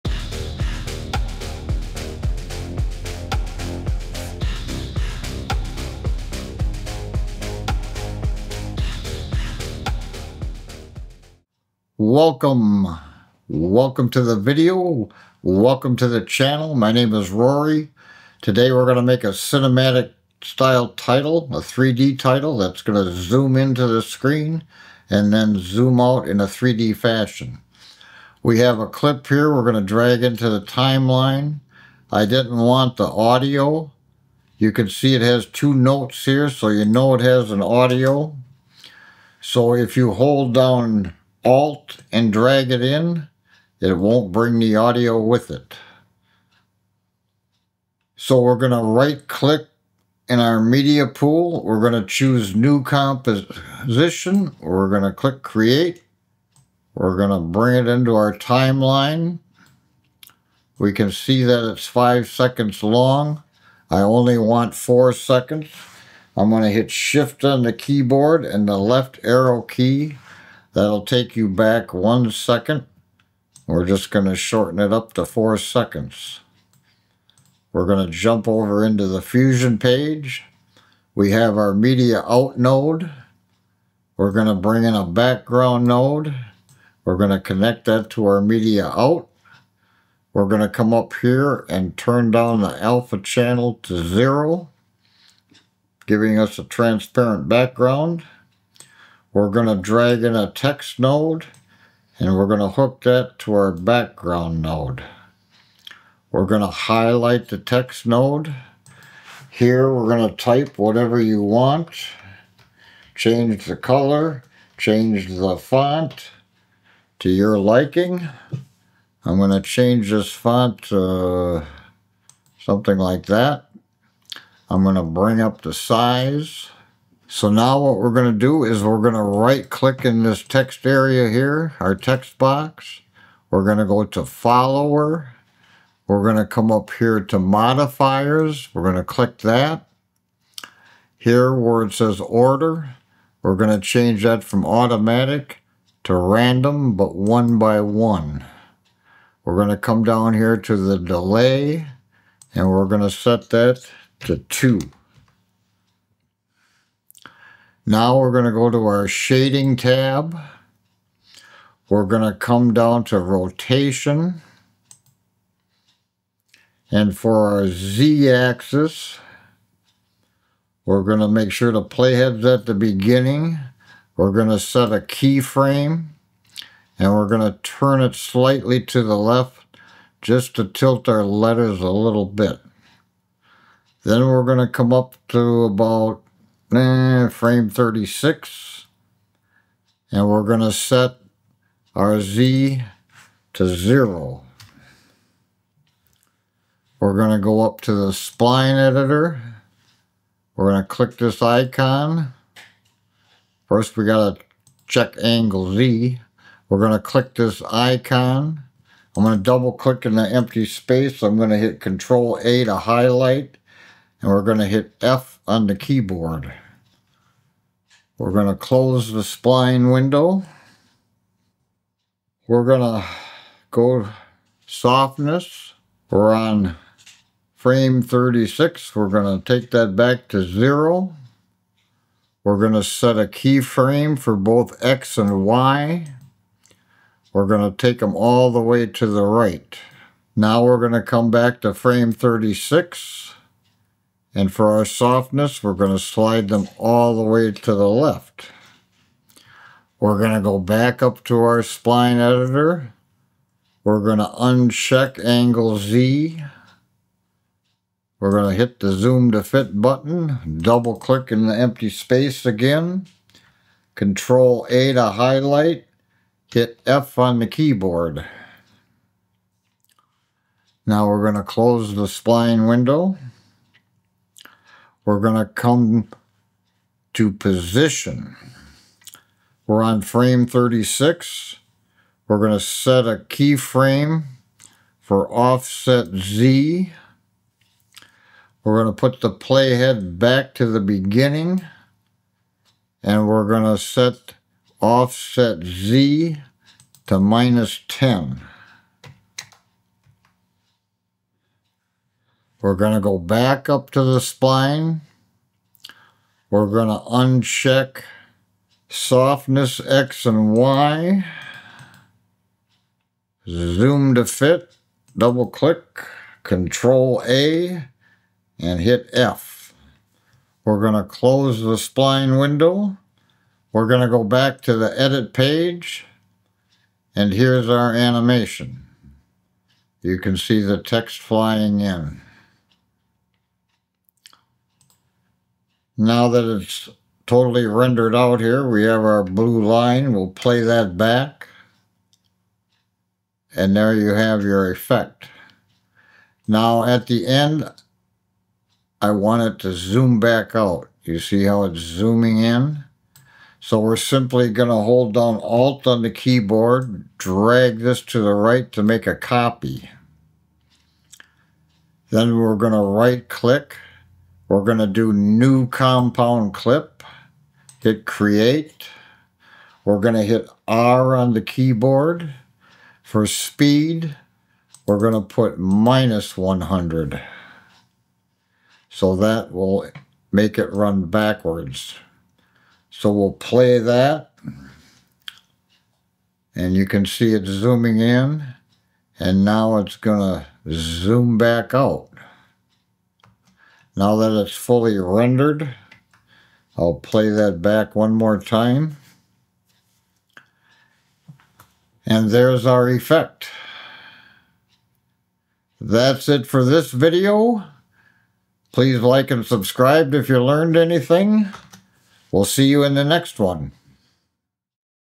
DaVinci Resolve: Epic 3D Cinematic Titles That Look Like Hollywood Movies
Learn how to create stunning 3D animated cinematic titles in DaVinci Resolve using the powerful Fusion page! This step-by-step tutorial will teach you professional techniques for designing eye-catching animated titles that will elevate your video projects.
🎬 What You'll Learn:

How to navigate DaVinci Resolve's Fusion page for 3D animation
Creating dynamic 3D text with depth and dimension
Animating your titles with smooth transitions
✨ Perfect for:

Video editors looking to upgrade their title game
Content creators wanting professional-looking intros
Filmmakers seeking cinematic title effects
Anyone wanting to master DaVinci Resolve's Fusion tools

🔧 Software: DaVinci Resolve (Free version works!)
⏱️ Skill Level: Beginner to Intermediate